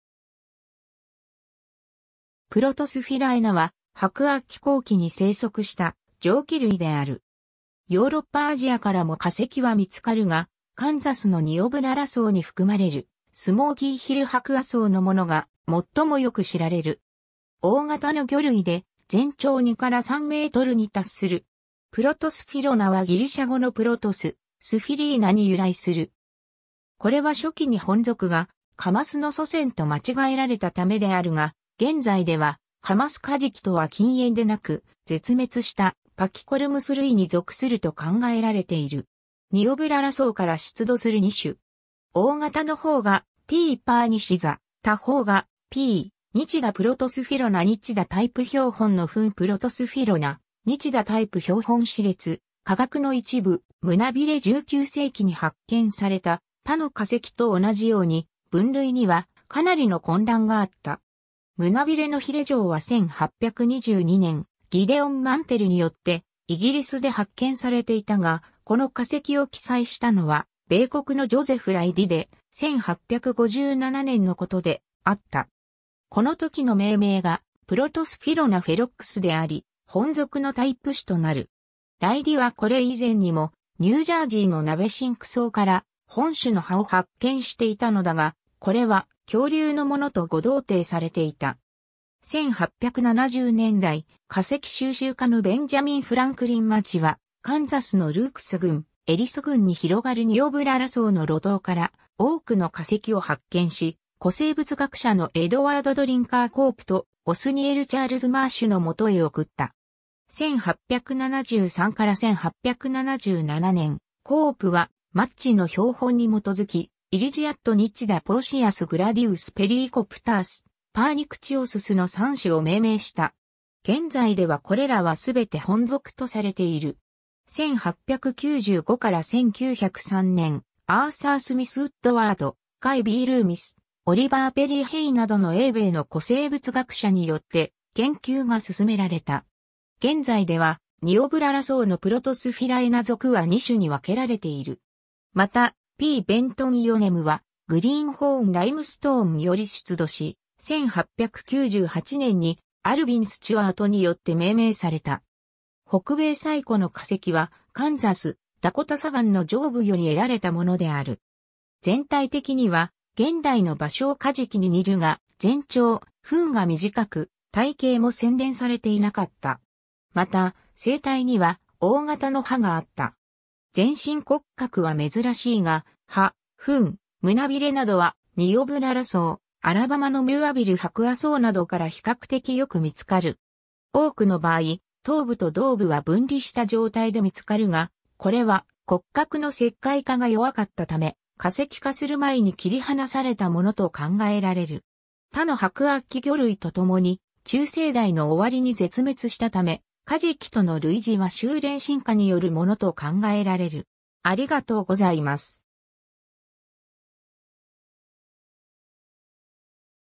プロトスフィラエナ
プロトスフィラエナ (Protosphyraena) は白亜紀後期（コニアシアン-マストリヒシアン）に生息した条鰭類である。
ヨーロッパ・アジアからも化石は見つかるが、カンザスのニオブララ層に含まれるスモーキーヒル白亜層（コニアシアン後期-カンパニアン前期）のものが最もよく知られる。
大型の魚類で、全長2-3 mに達する。
 Protosphyraena はギリシャ語の protos（"原始的な"）、Sphyraena（カマス属）に由来する。
これは初期に本属がカマスの祖先と間違えられたためであるが、現在ではカマス・カジキとは近縁でなく、絶滅したパキコルムス類に属すると考えられている。
 ニオブララ層から出土する2種。
大型（約3 m）の方がP. perniciosa 、他方がP. nitida Protosphyraena nitida タイプ標本の吻 (from Hay, 1903) Protosphyraena nitida タイプ標本歯列・下顎の一部・胸鰭 (from Hay, 1903) 19世紀に発見された他の化石と同じように、分類にはかなりの混乱があった。
胸鰭の鰭条は1822年、ギデオン・マンテルによってイギリスで発見されていたが、この化石を記載したのは米国のジョゼフ・ライディで、1857年のことであった。
この時の命名が Protosphyraena ferox であり、本属のタイプ種となる。
ライディはこれ以前にも、ニュージャージーのナベシンク層（マストリヒシアン）から本種の歯を発見していたのだが、これは恐竜のものと誤同定されていた。
 1870年代、化石収集家のベンジャミン・フランクリン・マッジは、カンザスのルークス郡・エリス郡に広がるニオブララ層の露頭から多くの化石を発見し、古生物学者のエドワード・ドリンカー・コープとオスニエル・チャールズ・マーシュの元へ送った。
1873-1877年、コープはマッジの標本に基づき、Erisichte nitida ・"Portheus" gladius ・"Pelecopterus" pernicciosus の3種を命名した。
現在ではこれらは全て本属とされている。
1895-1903年、 アーサー・スミス・ウッドワード (1895), F. B. Loomis (1900), Oliver Perry Hay (1903) などの英米の古生物学者によって研究が進められた。
 現在では、ニオブララ層のプロトスフィラエナ属は2種 (P. nitida ,P. perniciosa) に分けられている。
また、P. bentonianum は Greenhorn Limestone（セノマニアン後期）より出土し、1898年にAlbin Stewart によって命名された。
北米最古の化石は、カンザス、ダコタ砂岩の上部（セノマニアン中期）より得られたものである(Everhart, 2005; p. 91)。
 全体的には現代のバショウカジキに似るが、全長・吻が短く、体型も洗練されていなかった。
また、成体には大型の歯があった。
全身骨格は珍しいが、歯・吻・胸鰭などは、ニオブララ層・アラバマのMooreville白亜層などから比較的よく見つかる。
多くの場合、頭部と胴部は分離した状態で見つかるが、これは骨格の石灰化が弱かったため、化石化する前に切り離されたものと考えられる(Everhart, 2005; p. 93)。
他の白亜紀魚類と共に中生代の終わりに絶滅したため、カジキとの類似は収斂進化によるものと考えられる。